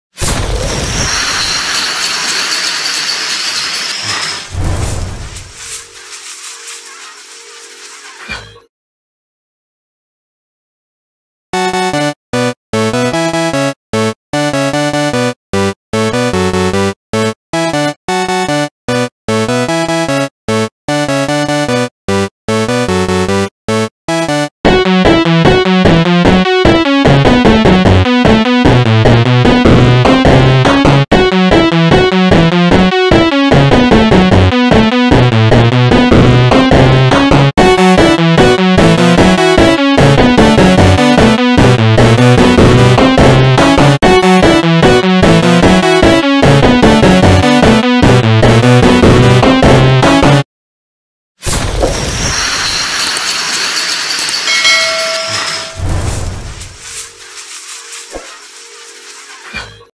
BIG NUMBERS 520 AND 1024?!!!
ENJOY THE NEW RETRO BLOCK BANDS!

Chapters:
0:00 Intro!
0:09 Retro 520 AND 1024 EACH SOUND?!
0:37 520 AN 1024 TOGETHER!!